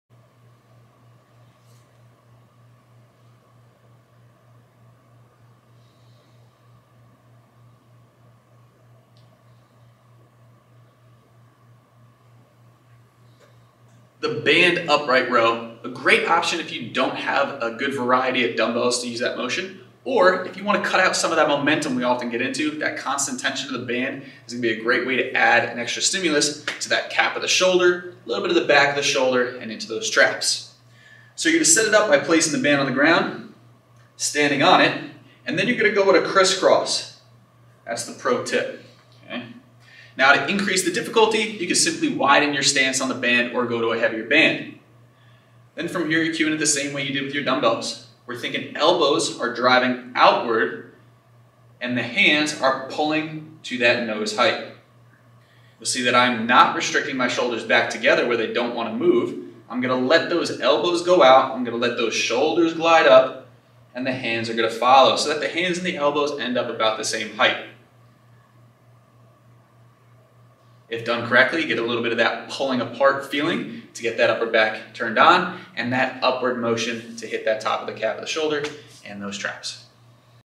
Band Upright Row
This video shows you how to perform a Band Upright Row with a full demonstration of the movement complete with in-depth cues and best practices. If you’re a beginner or advanced and you want to master the movement, this video will show you how in just a couple of minutes.

AND

If you want to work with one of our coaches or trainers in person or online, visit our